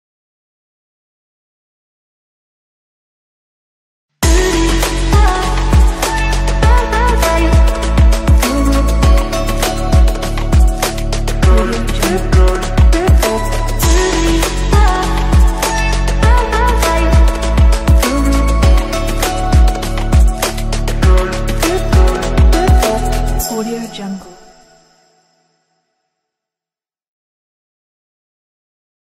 Book Promo for After Effects 2023
Included 7 Text placeholders and 1 Logo placeholder.

You can use After Effects CS6 or upper.

Full HD (1920×1080) resolation. Duration 25 sec.

All in After Effects and no need plug-in.

Audio and preview images are not included.

Fast render.